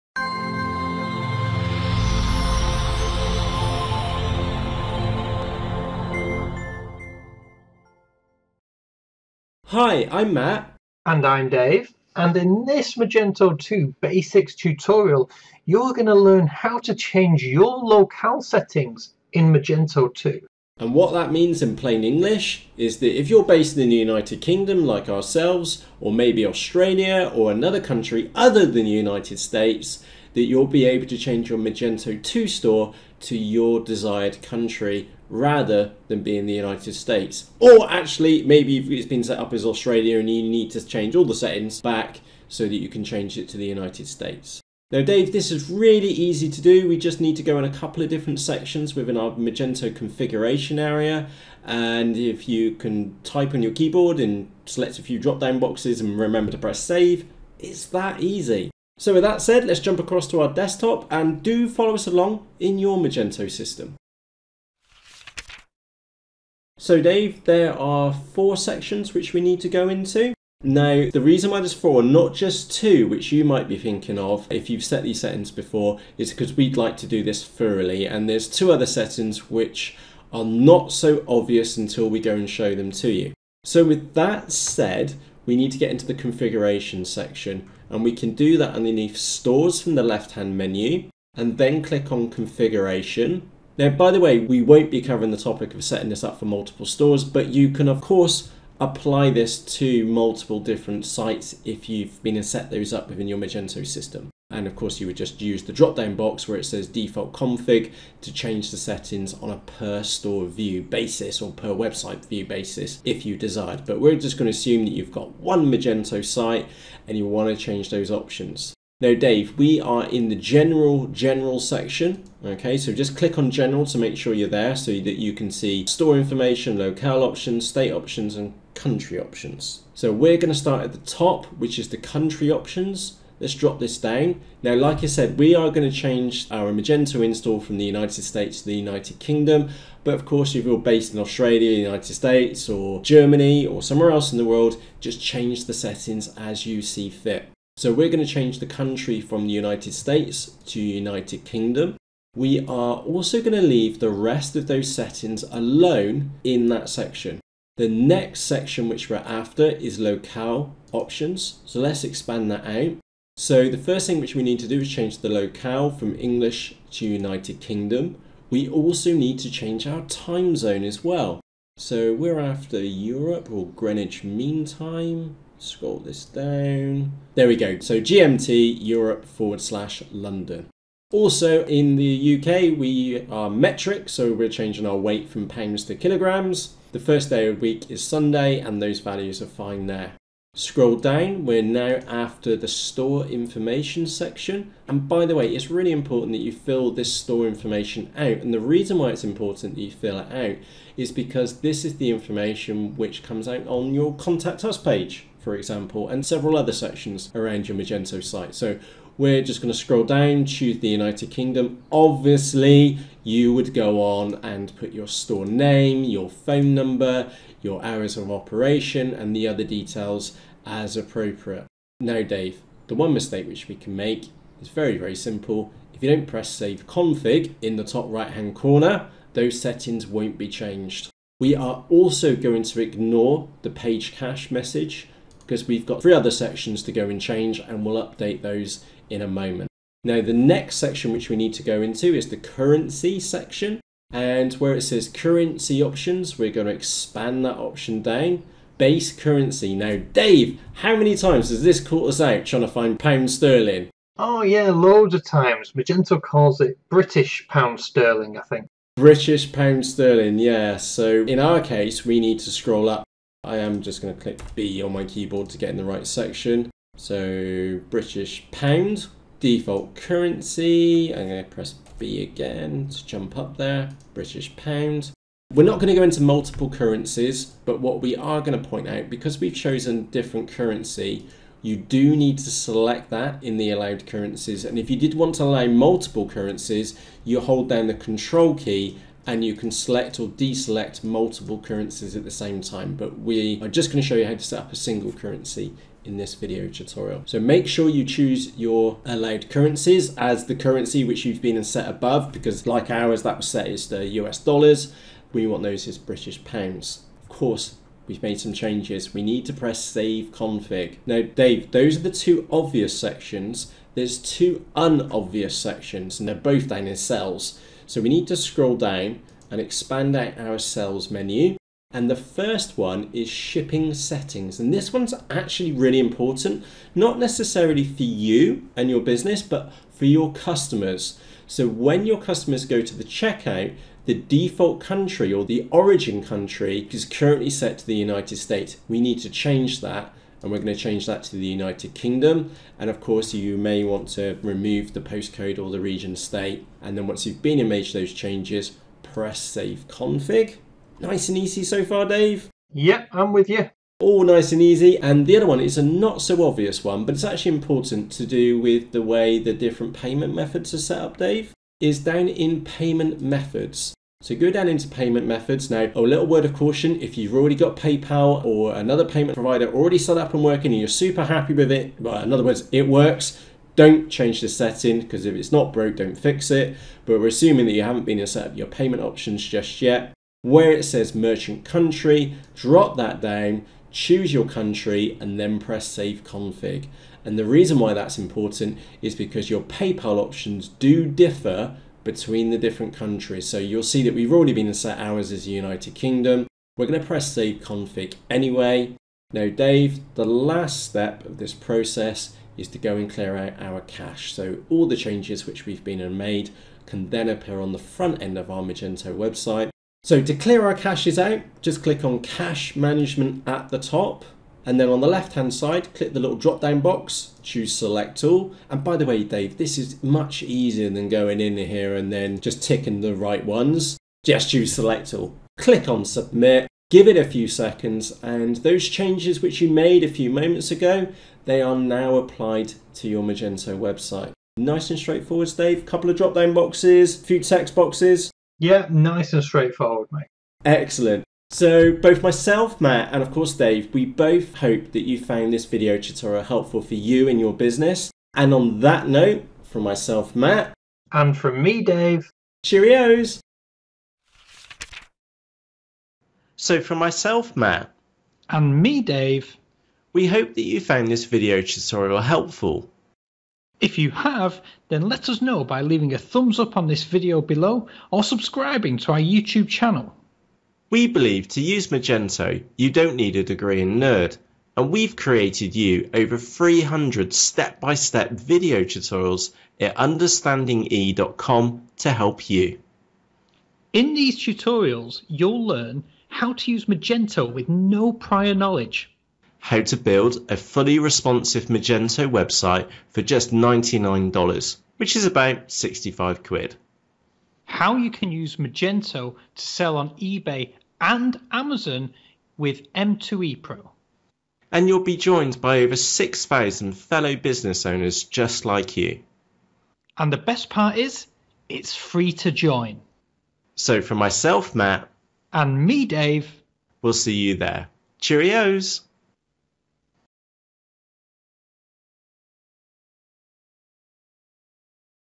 How to Change your Location/Locale Settings in Magento 2
In this tutorial we will show you how to change your Locale Settings in Magento 2 based on the location of your business.

Note that this tutorial is going to use the United Kingdom as an example of a Magento store location, so just refer to your own country of residence as you follow along.

Step 1. Set up Your General Locale Settings

- From your admin panel, go to the Stores menu on the left-hand side then click on Configuration.

- Once you’re in the Configuration section, click on the General tab on the left-hand side then expand the first section called Country Options by clicking on the down arrow to its right.

- Set the Default Country to United Kingdom then scroll down and expand the section called Locale Options.

- Set your Timezone accordingly, so in this case it would be Greenwich Mean Time (Europe/London).

- Next, choose English (United Kingdom) from the Locale drop-down selections, and then change the Weight Unit from lbs to kgs.

- After that, expand the Store Information section then fill out all the applicable fields as the information you put in here will appear on your website.

- Once that’s done, press Save Config in the top right-hand corner.
Step 2. Set up Your Local Currency

- Click on the Currency Setup on the left-hand side then expand the first section labelled Currency Options.

- From the drop-down selections for Base Currency and Default Display Currency, choose British Pound.

- Choose the same currency for the Allowed Currencies option; to allow multiple currencies in your store, hold down the ctrl key on your keyboard then select your preferred currencies.

- Once you’re finished setting up the currency(ies) in your store, press Save Config at the top right.

Step 3. Set up Your Shipping Settings

- On the left-hand side, click on the Shipping Settings underneath the Sale section.

- Expand the first section called Origin then set the Country to United Kingdom so that your customers can see the country you’re based in when they reach the checkout of your website.

- Remove the ZIP/Postal Code then click on Save Config in the top right-hand corner.

Step 4. Set Your Merchant Country

- Click on Payment Methods, which is underneath the Sales section, on the left-hand side.

- If you already have PayPal or another payment method set up in your Magento system, and it works just fine, leave it as it is.
However, there is one important thing that you need to do.

- Choose your country from the drop-down selections for Merchant Country then press Save Config at the top right.

- That’s a simple but very important step as the settings for PayPal would differ from one country to another.

Step 5. Clear the Caches

- Click on the Cache Management link that you see in the pop-up message at the top of your screen.

- Next, choose Select All from the drop-down box on the left-hand side, press Submit and then wait for the page to refresh.

Matt & Dave